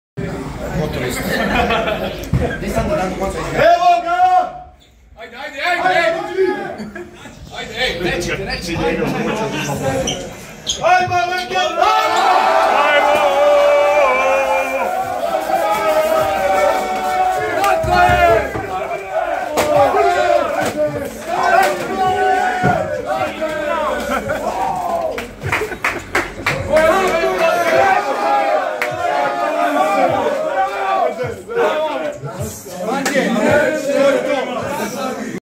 Krka squad celebrate their 2nd NLB ABA League 2 trophy
Check out the post-game celebration by the Krka squad from the locker room, as they have grabbed their second NLB ABA League 2 trophy.